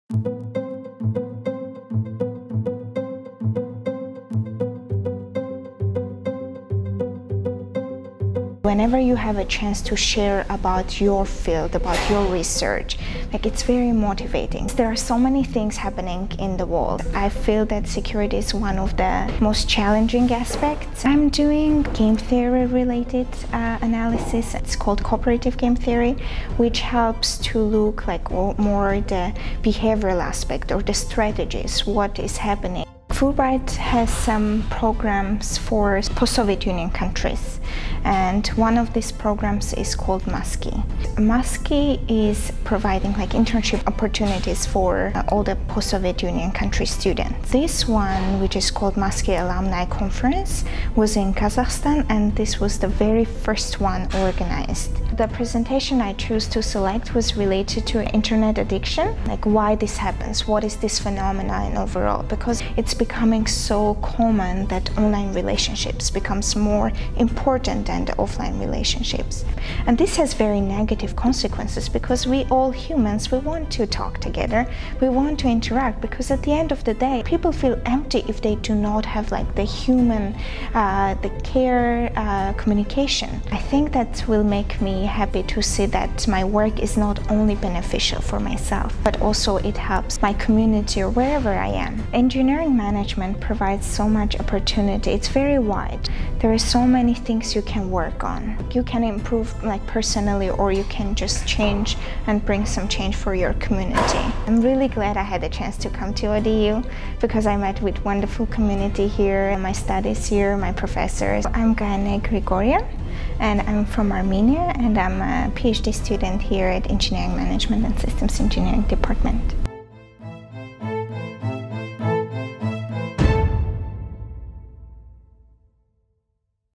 Engineering Management Ph.D. Student/Fulbright Scholar, Presents Game Theory Research in Kazakhstan
Gayane Grigoryan, a Ph.D. Engineering Management student and Fulbright Scholar (alumna) from Armenia, received an all-expense-paid trip to attend the inaugural Edmund Muskie Alumni Conference held in Kazakhstan's newly-renamed capital Nur-Sultan (formerly Astana). Grigoryan presented research related to internet addiction and cooperative game theory, by focusing on the human aspect of the security and analyzing what types of coalitions will form, the joint actions groups take, and the resulting collective benefits or detriments.

Nur-Sultan Photos: Indy-Guide